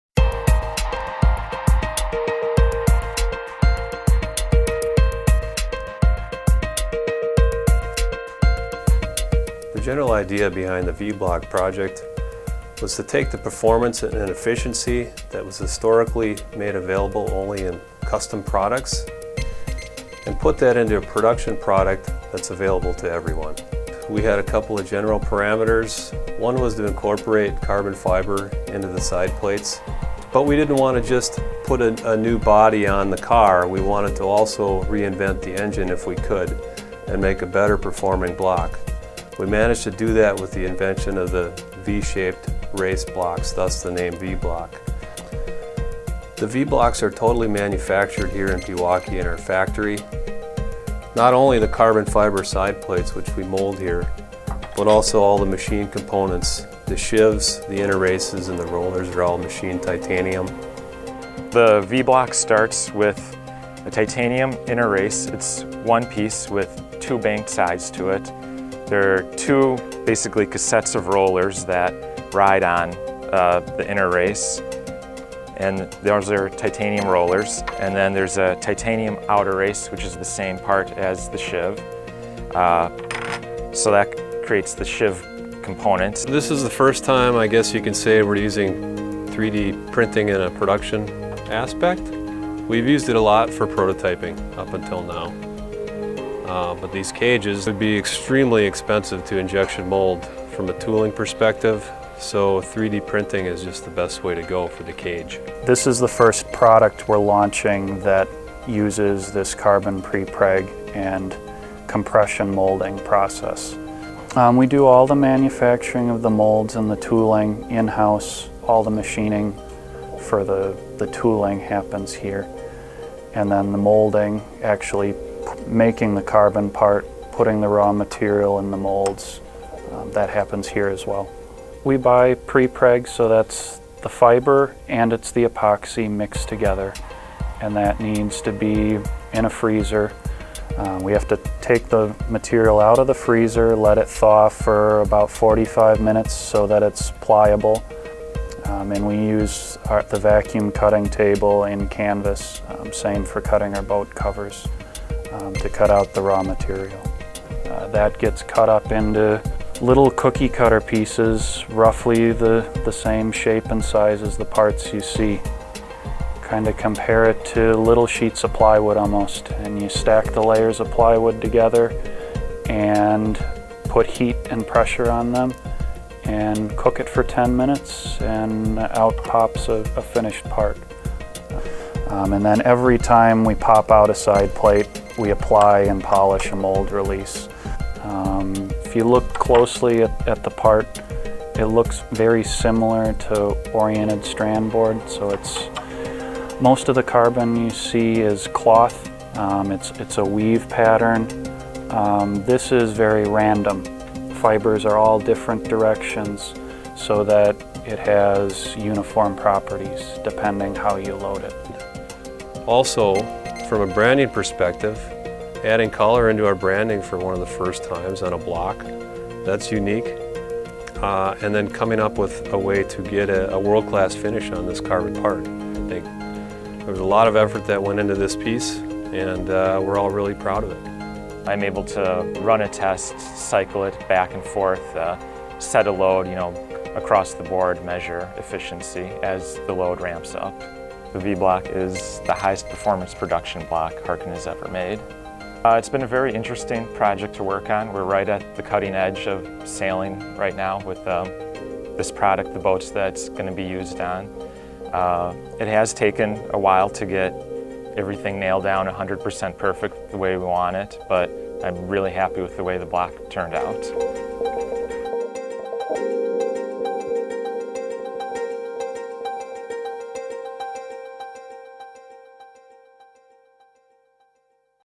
Harken V Block: How It's Made
The V™ block is the highest performing, most efficient block Harken has ever offered. Engineers chose extremely strong, extremely lightweight materials to build block components-3D molded carbon sideplates, titanium races, and captive titanium roller bearings. Then they combined the axial and thrust loads into a single bearing set of angled titanium rollers. The result is a 12% decrease in the number of unique components for an incredibly powerful, but simple lightweight block that surpasses any other block we've ever tested in efficiency at high loads, while spinning freely at low loads for smooth easing in light air.

"To eliminate lead times for customers, we decided to transform our highest performing Grand-Prix blocks. Where previously they have always been custom products, now they will be production products available on-the-shelf," says Mark Wiss, Director of New Business Development. "We increased the high-load efficiency and greatly improved low-load release responsiveness. We named it the V block because the angled inner races and sheave surfaces are 'V' shaped."

V blocks are easy to disassemble for inspection and maintenance. They are available in four (4) sizes dedicated to maximum working loads of 1500 kgf, 3000 kgf, 5000 kgf, 6500 kgf.

Sideplates are also available in anodized aluminum.